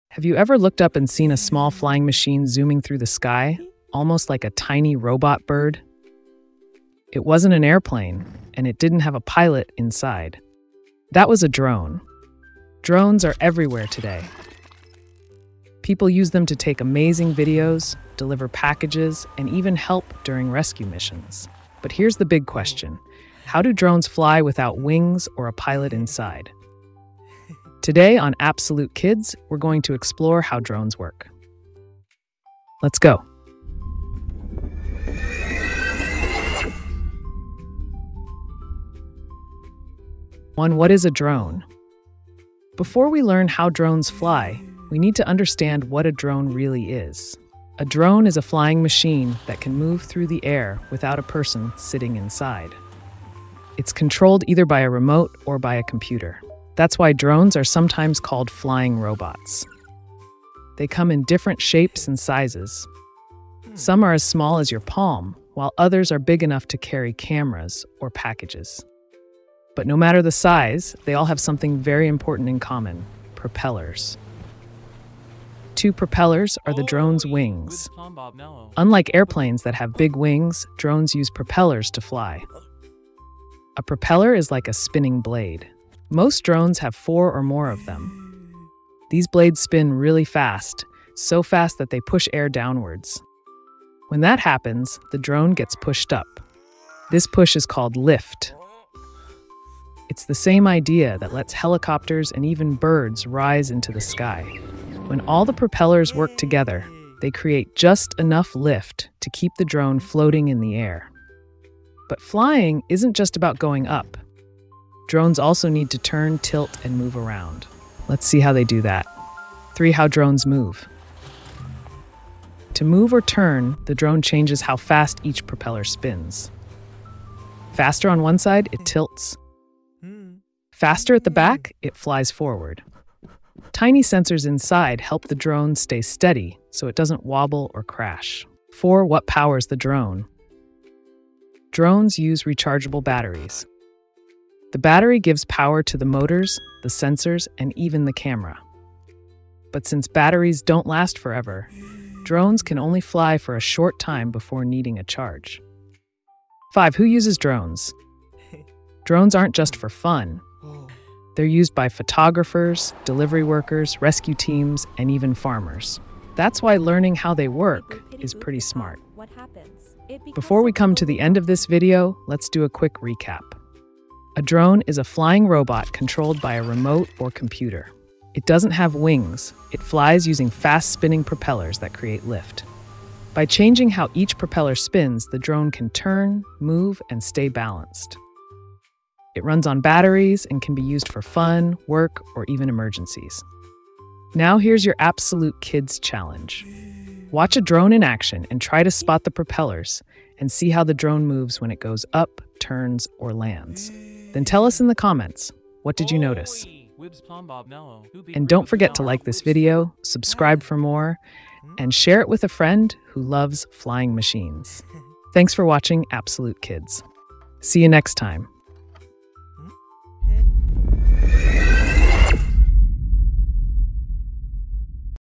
How Drones Fly: Easy Science for Kids | STEM Learning with Appsolute Kids
Ever wondered how those cool flying machines called drones zoom through the sky with no pilot inside? In this fun and educational video, Appsolute Kids breaks down the science of drones for kids in the most exciting way possible.

✔️ What is a drone?
✔️ How do propellers make drones fly?
✔️ How do drones move and stay balanced?
✔️ What powers drones?
✔️ Who uses drones and why?

Perfect for curious kids, classrooms, science lovers, and tech beginners. Whether you’re a parent, teacher, or just love flying machines, this video is your perfect intro to how drones work. Plus, we’ve got a cool challenge at the end for our junior aviators.